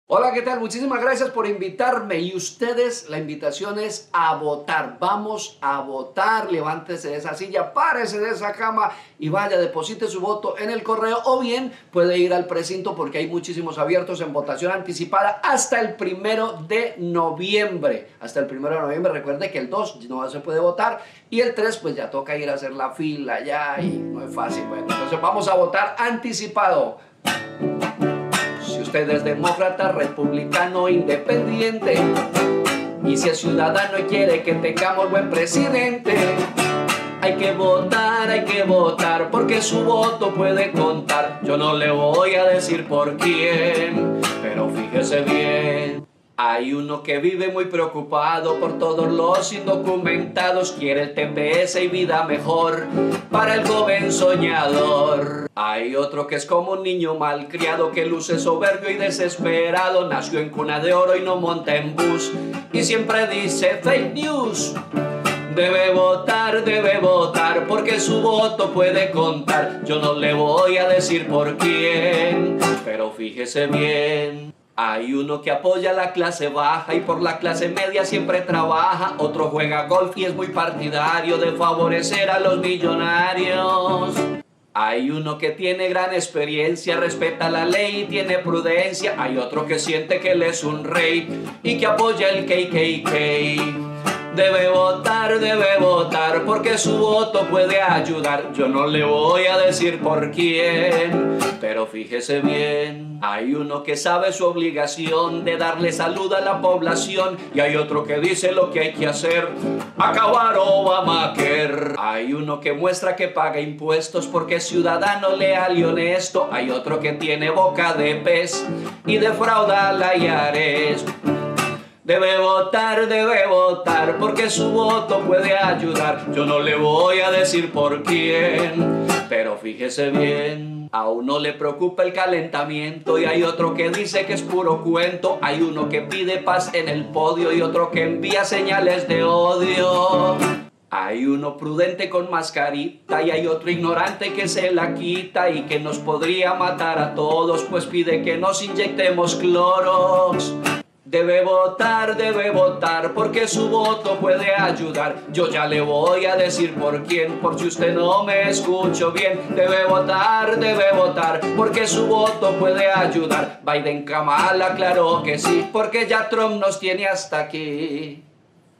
¿Biden o Trump?
El comediante colombiano Salud García nos dice "Hay que votar. Yo no voy a decirle por quién, pero fíjese bien"